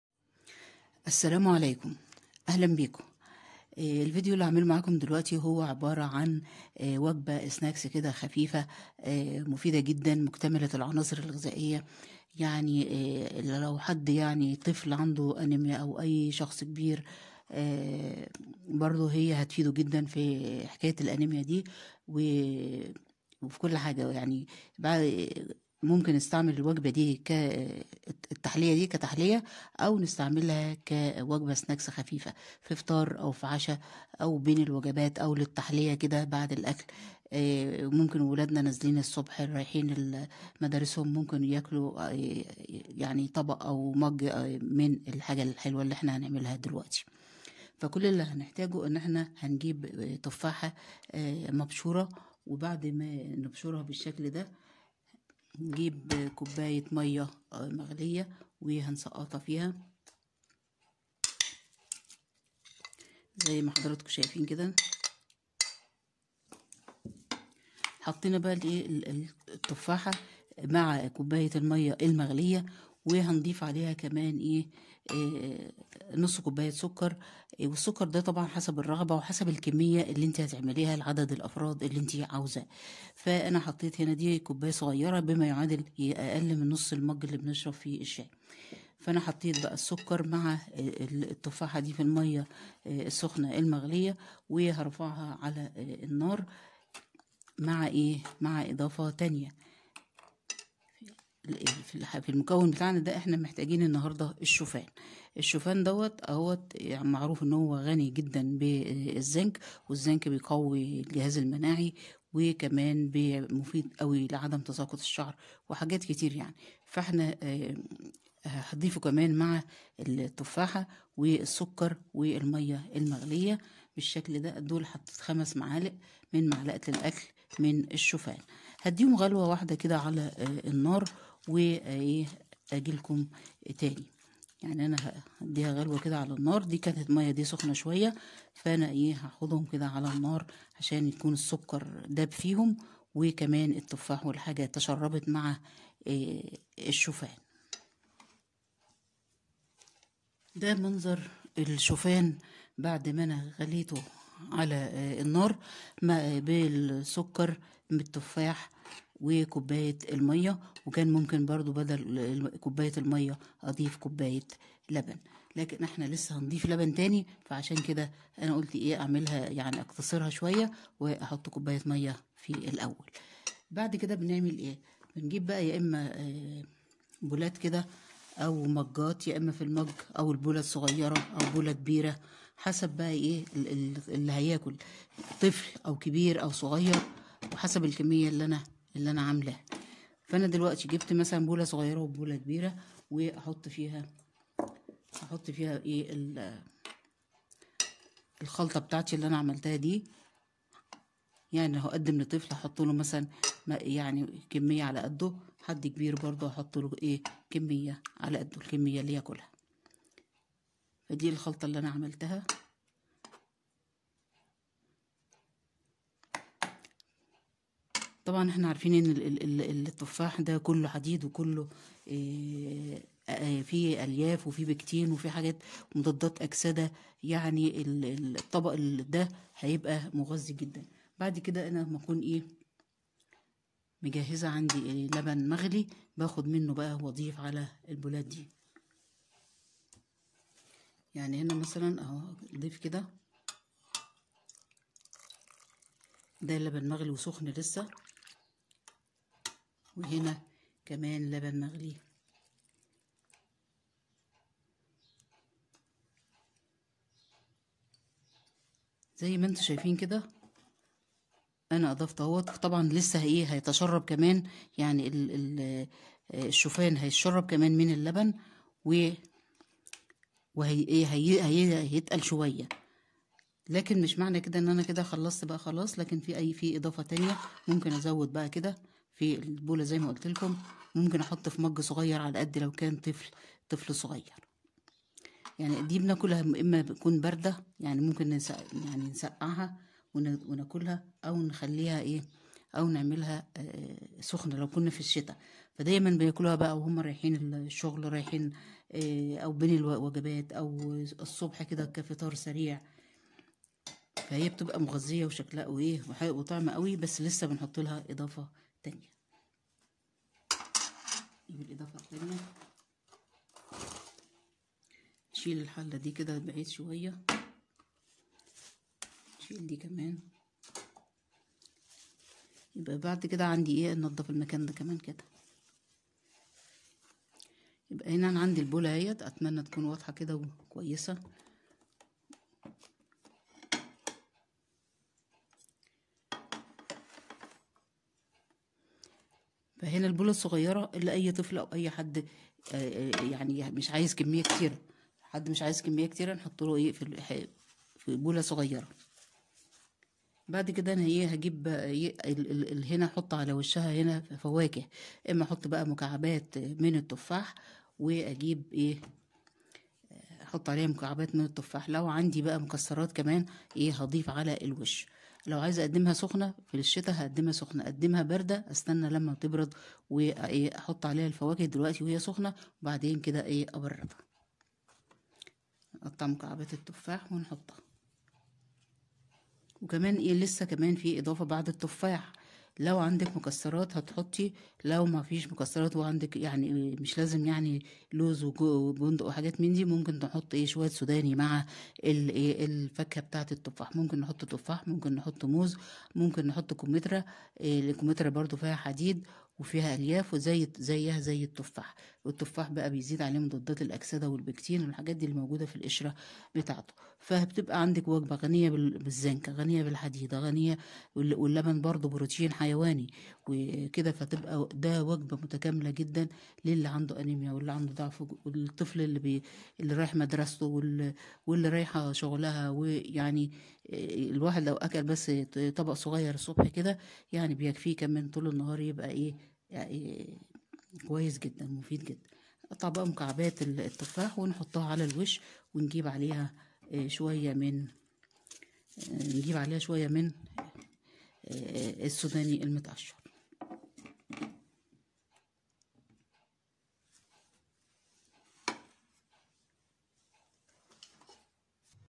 حلا الشوفان بالفواكه وجبه اسناكس فطار او عشاء مفيده للانيميا @ عزه زيدان
حلا الشوفان بالتفاح     وجبه فطار    او وجبه عشاء    تعالج الانيميا    وفقر الدم
المقادير   كوب ماء نصف كوب شوفان  نصف كوب سكر   يغلي علي النار   يعبأرفي بولات او كا سات   يضاف لبن حليب وقطع تفاح   ومقدار من المكسرات او السوداني علي الوجه  تقدم لكل افراد الاسره @ عزه زيدان